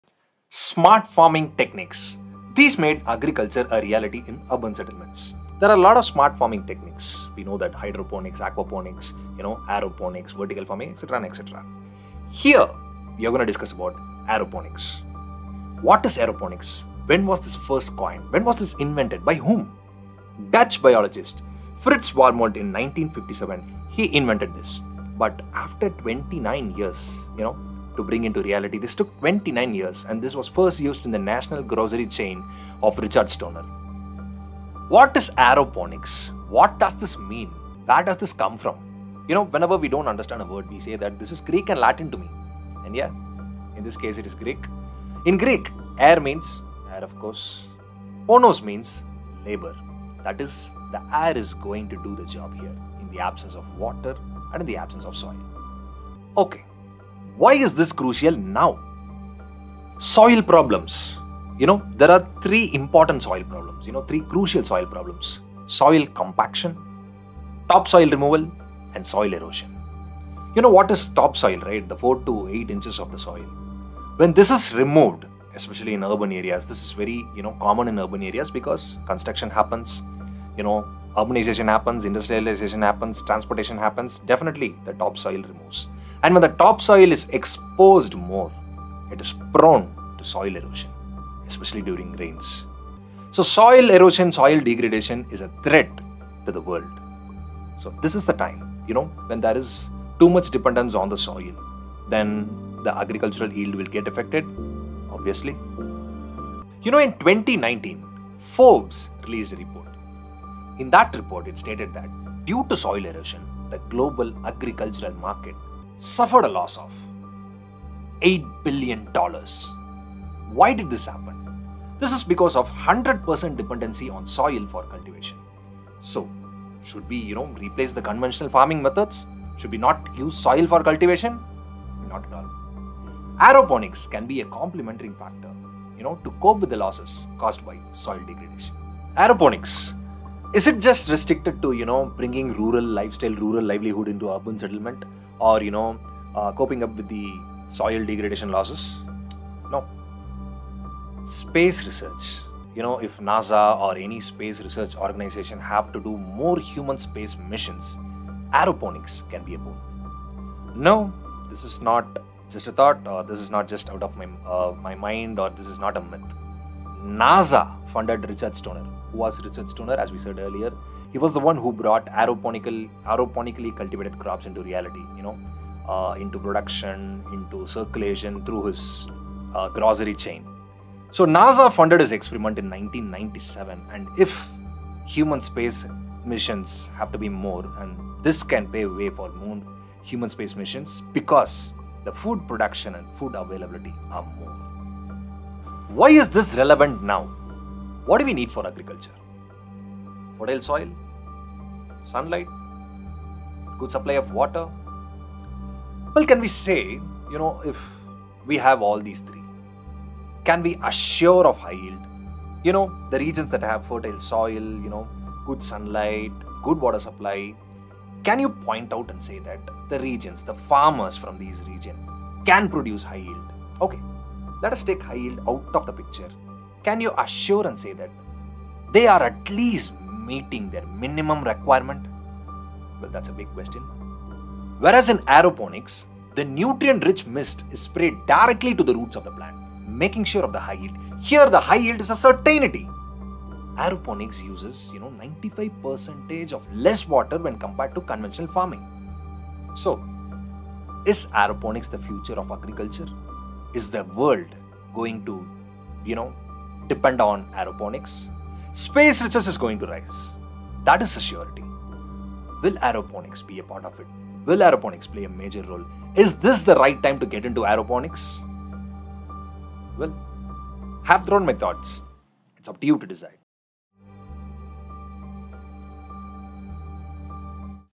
What is Aeroponics? Is this the Future of Agriculture?
The term aeroponics was originally coined by the Dutch biologist, Frits Warmolt Went in 1957 but it was a full 29 years before the first aeroponically grown food was sold in a national grocery chain by Richard Stoner.
Whenever we don't understand a word or sound new, it is definitely extracted from Greek or Latin, Greek in this case; aer (air) and ponos (labour) means working air.
We all know that from our previous video, visit the video if you haven’t seen it, aeroponics doesn’t need soil or water or very little water.

Will aeroponics become the future of agriculture? Space research is definitely going to raise, will aeroponics be a part of it? Will this become inevitable?
Is this the right time to start this methodology?
Well, that’s for you to decide.